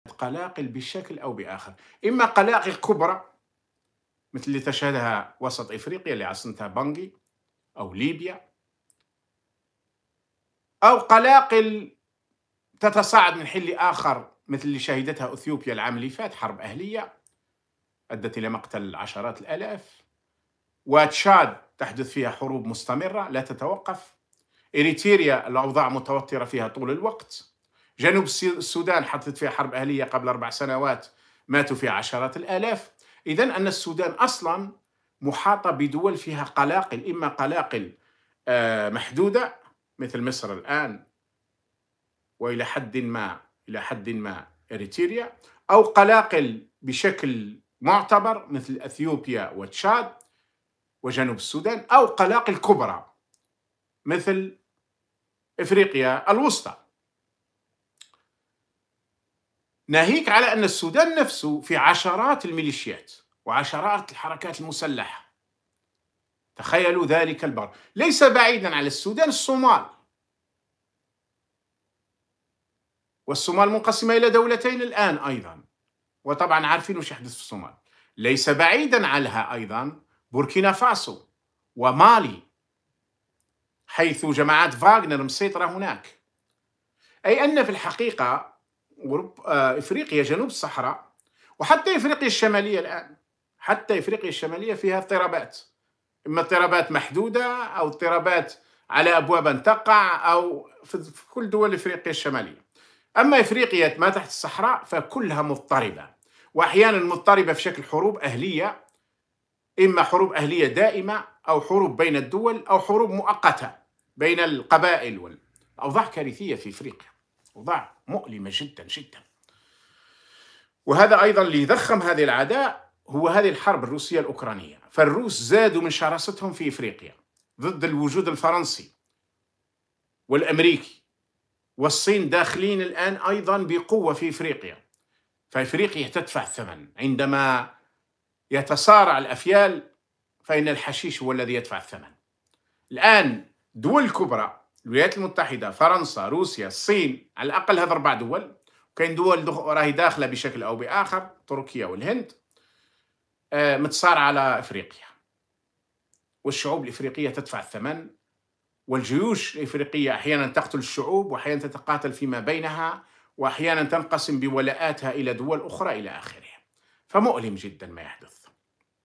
إفريقيا: أوضاع مضطربة وأزمات مستمرة..."عندما يتصارع الأفيال يموت العشب" !
محمد العربي زيتوت 18 أفريل 2023